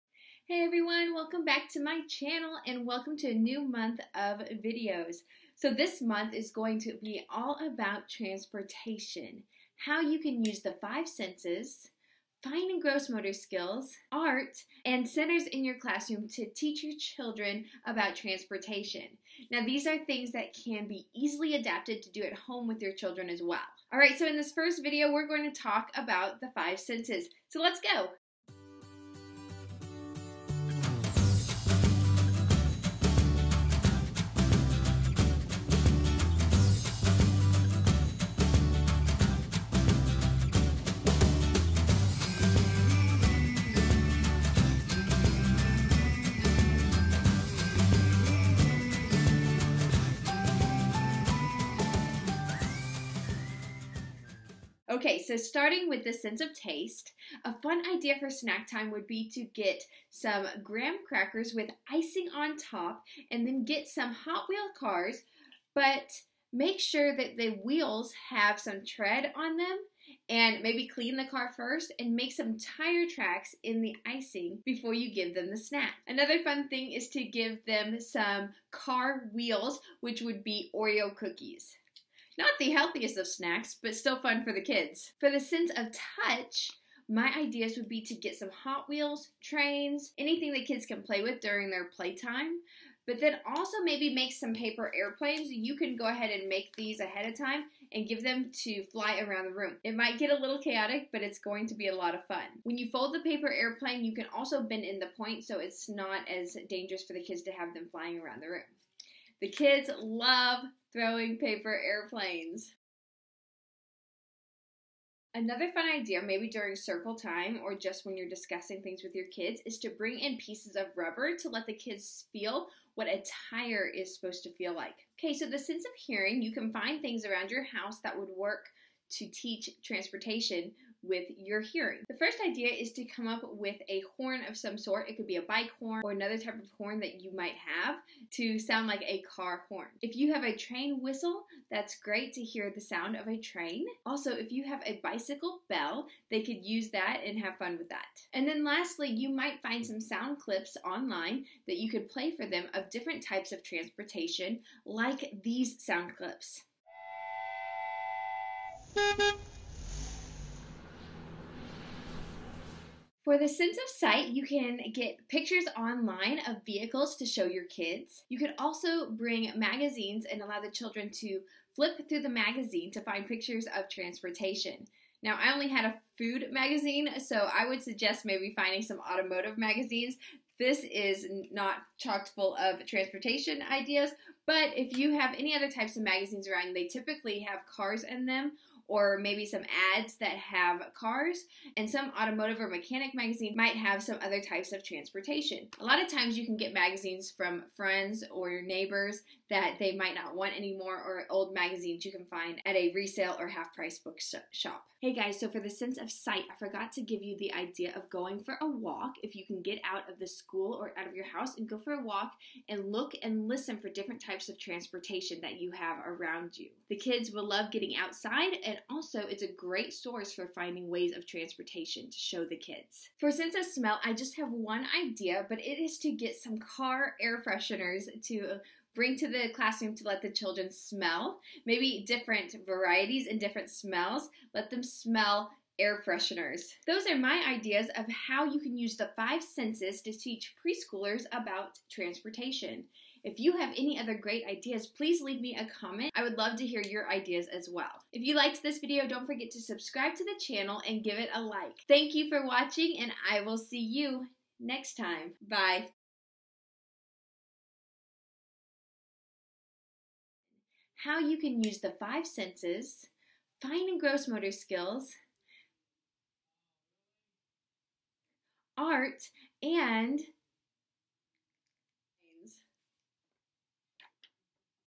LEARNING ABOUT TRANSPORTATION USING THE FIVE SENSES!!! Preschool transportation activities part 1!!!
This is the first video talking about transportation themed activities for preschoolers. This video talks about exploring transportation using the five senses.

Hot Wheels Cars
Train Whistles
Transportation Flashcards
Car Air Fresheners

Index:
00:00 - 00:55 Intro
00:55 - 01:25 Taste
01:25 - 02:04 Touch
02:04 - 02:43 Hearing
02:43 - 03:44 Sight
03:44 - 04:20 Smell
04:20 -04:35 Outtakes